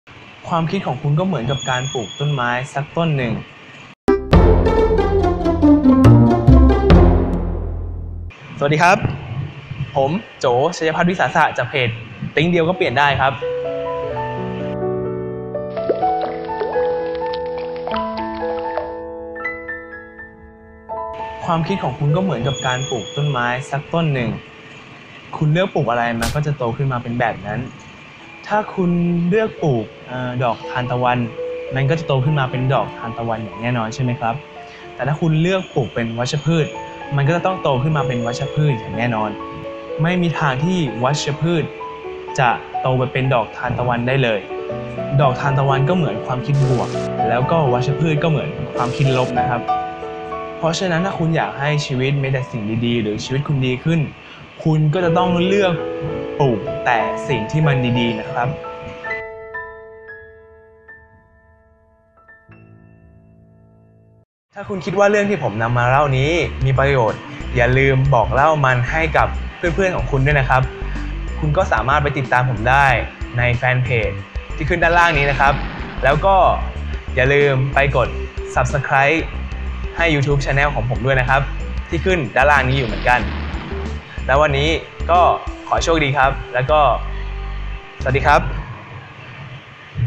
ความคิดของคุณก็เหมือนการปลูกต้นไม้
ก่อนคุณจะปลูกอะไรลงไปในความคิดของคุณ คิดให้ดีๆ
➤ เข้ามาดูเรื่องราวเพิ่มเติมได้ที่นี่
จะได้ไม่พลาดเรื่องราวดีๆนะ
คลิปใหม่ๆออกทุก วันพุธ &
ครั้งหน้าอยากให้ผมพูดเกี่ยวกับเรื่องอะไร คอมเม้นไว้ได้เลยนะครับ